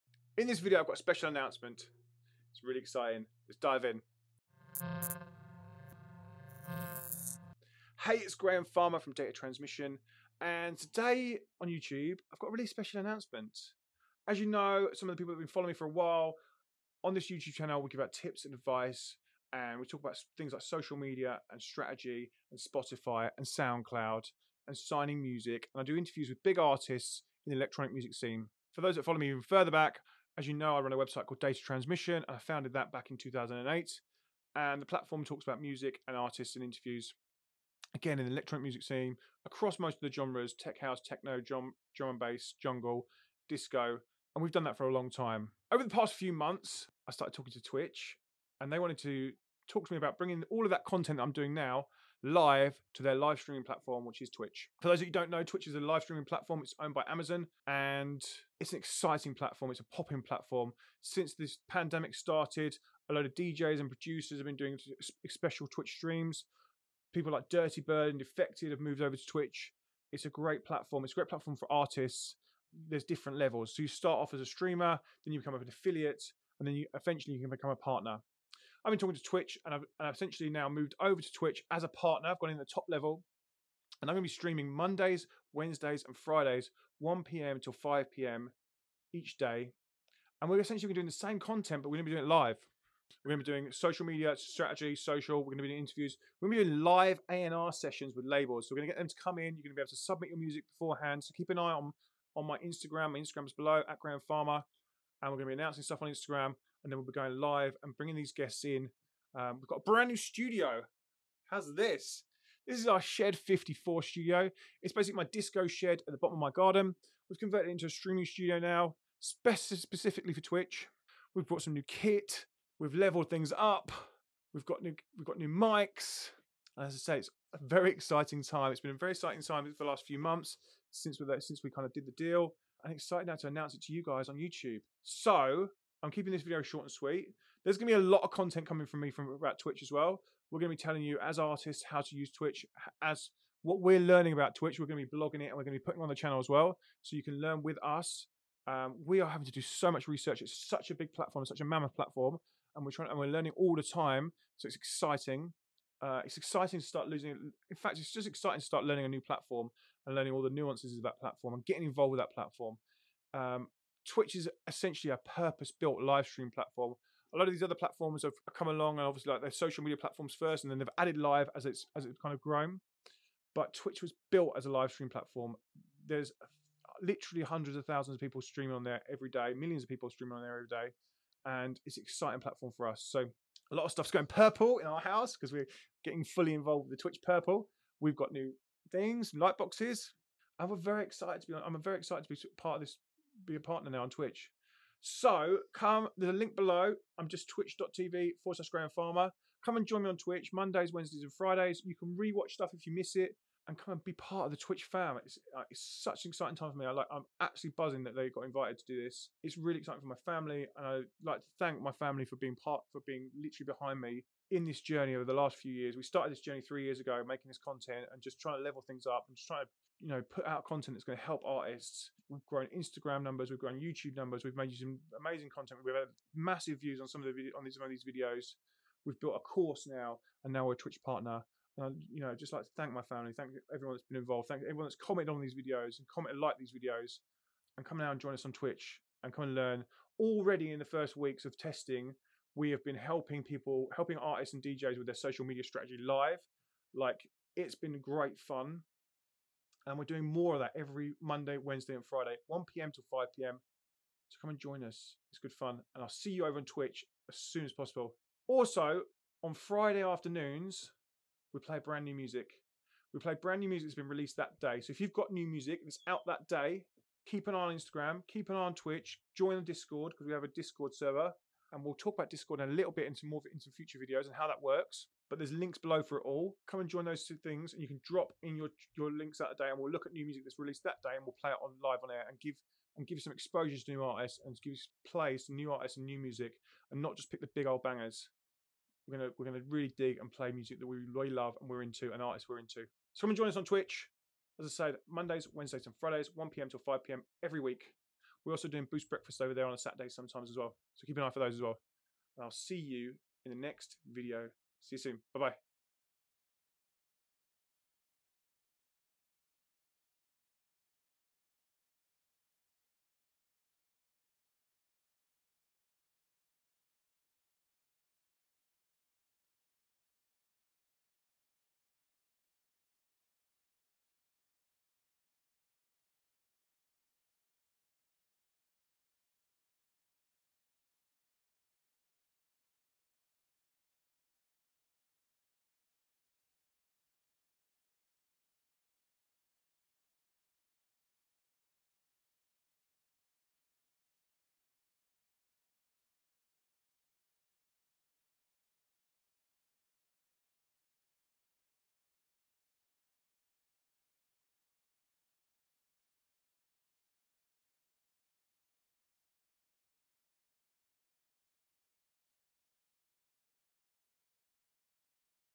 I'm a Twitch Partner!
I'm excited to announce I'm a twitch partner, I'm bringing all the content that usually happens on my Youtube channel to the LIVE space on Twitch.  I'm streaming Monday, Wednesday and Fridays from 1pm-5pm that's UK time.

I'm doing LIVE interviews with Artists, DJs, Producers, Label Owners, Music Business heads to get you more information and the Blueprint to navigate the music business as a DJ / Producer.

Your comments mean the world to us, its driving us on to make more content, so please take a second and say ‘Hey’ .

Data Transmission Producer Tools

Data Transmission Filming Equipment